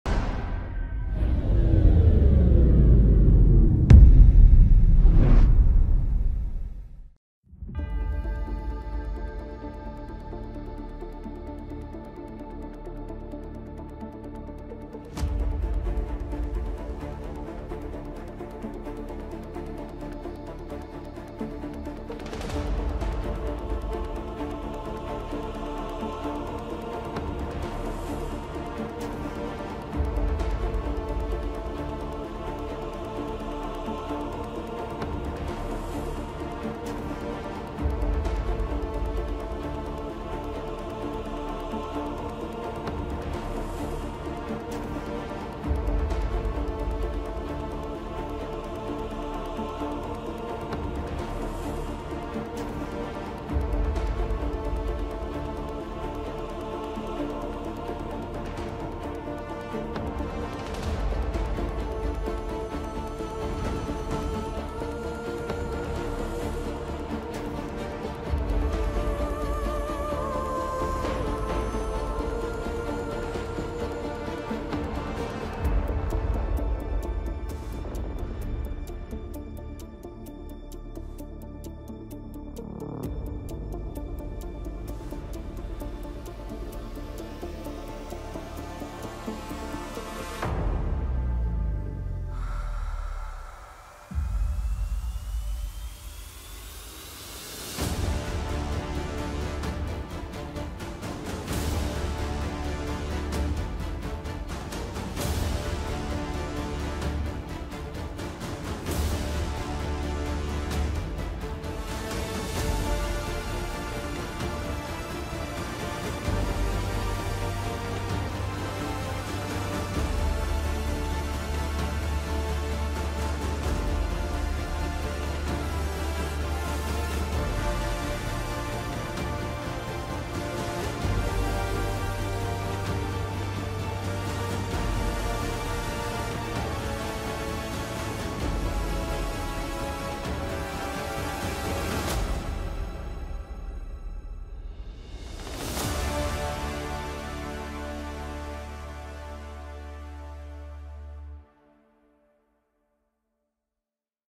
Azerbaijan thwarts another Armenian armed provocation on the border - Caliber News English
On February 12, the Armenian Armed Forces shelled the Azerbaijani military positions from the position in the direction of Nerkin-And settlement of Gafan district. As a result of the provocation, a serviceman of the Azerbaijani State Border Service soldier Khalilzade Parviz was wounded. The units of Azerbaijan’s State Border Service carried out "Operation Revenge" in response to the February 12 provocation of Armenian armed forces.